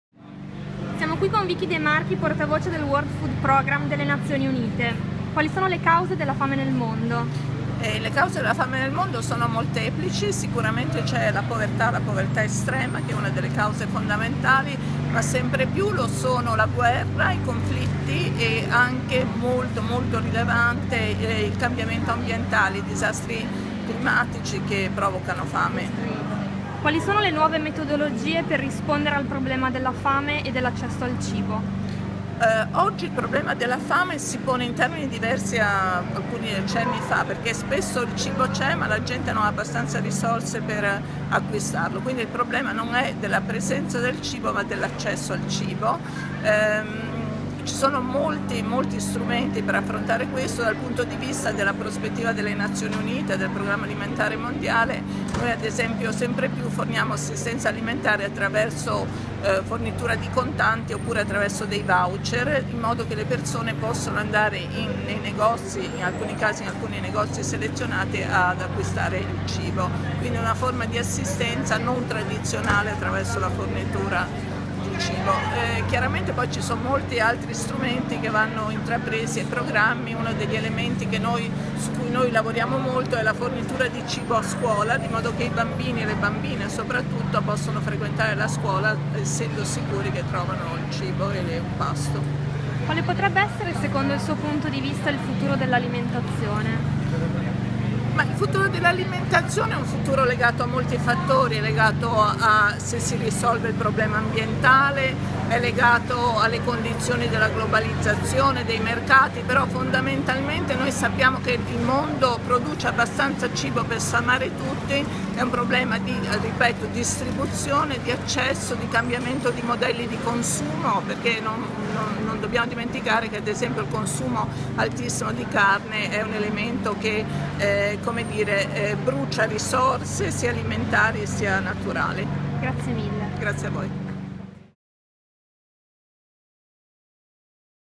Nuove frontiere dell'assistenza alimentare | Vichi de Marchi
Si è parlato di Nuove frontiere dell’assistenza alimentare venerdì 15 aprile in un incontro in Università Cattolica, promosso dal dipartimento di Scienze politiche. Ospite speciale Vichi De Marchi, che abbiamo intervistato, portavoce per l’Italia del World Food Programme delle Nazioni Unite. Hanno partecipato all’evento anche il preside della facoltà di Scienze politiche e sociali Guido Merzoni, il direttore del Dipartimento Massimo De Leonardis e la docente di Diritto internazionale Marinella Fumagalli.